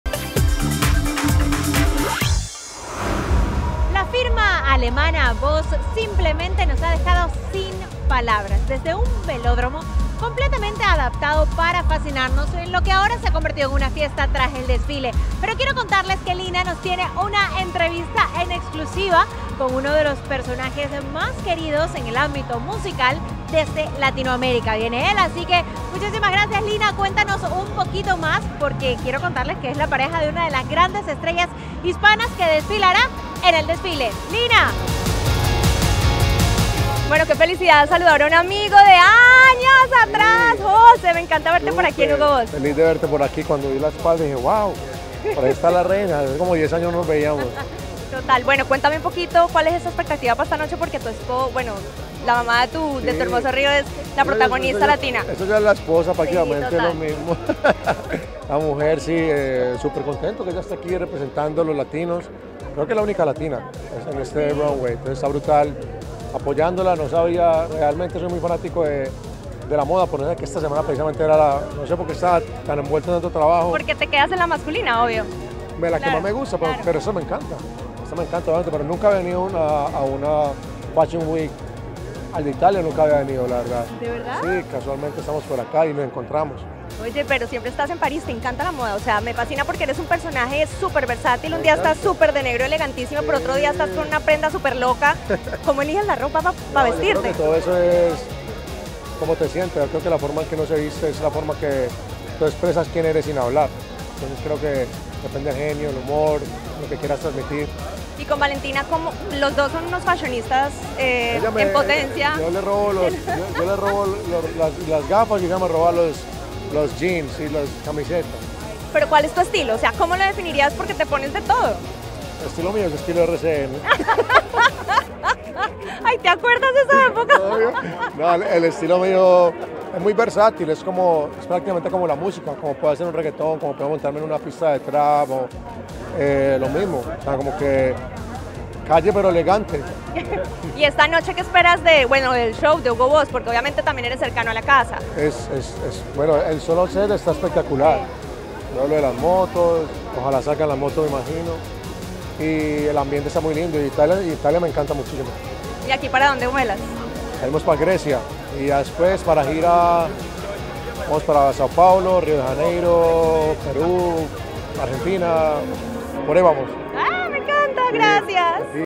¡En exclusiva! J Balvin confiesa qué es lo que se roba del clóset de su pareja
Revelamos los secretos del estilo de J Balvin directamente desde el desfile de BOSS en Milán. ¡Prepárate para risas, anécdotas y revelaciones impactantes que no puedes perderte!

índice
00:00 Intro
00:36 Valentina Ferrer, pareja de Balvin, desfiló en la pasarela de BOSS
00:52 J Balvin saluda a Kim Kardashian en el desfile de Dolce & Gabbana
02:20  Balvin expresa sus sentimientos a través de la moda